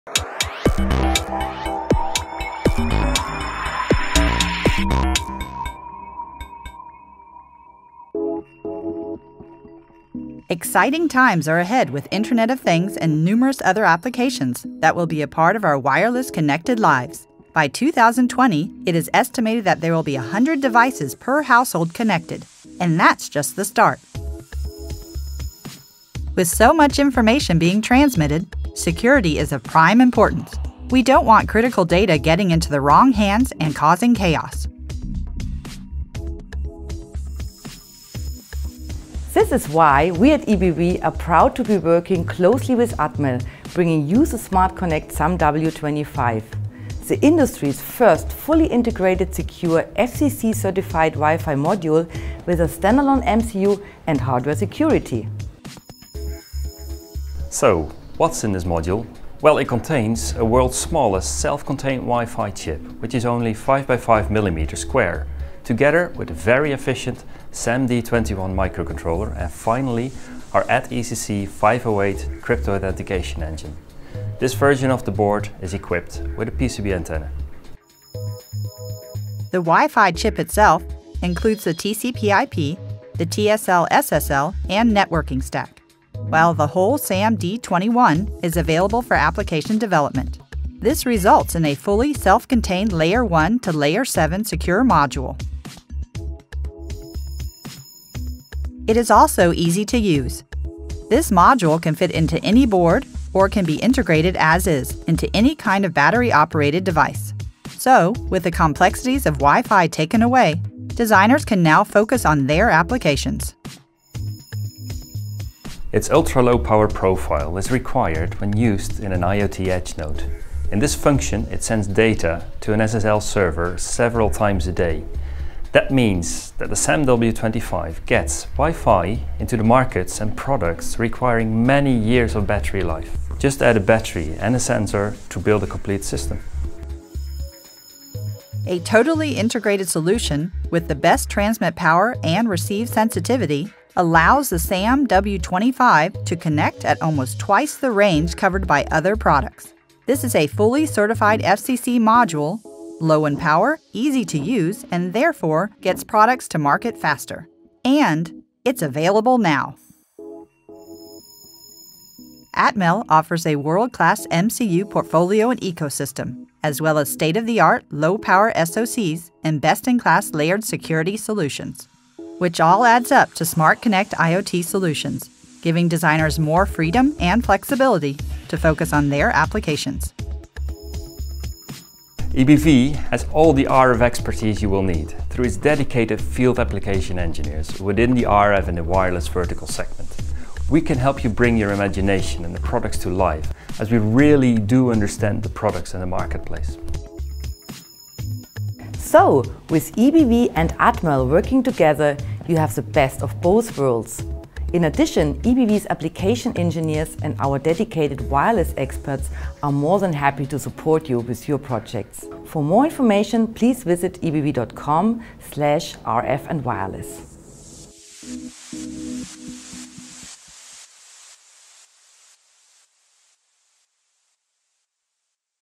SmartConnect - Introducing the SAMW25 Standalone Edge Node Module from Atmel & EBV
Exciting times are ahead with IoT and numerous other applications that will be part of our wireless connected lives.  By 2020 it is estimated that there will be 100 devices per household connected. And, that's just the start. With so much information being transmitted, security is of prime importance. So we bring to you the SmartConnect SAMW25.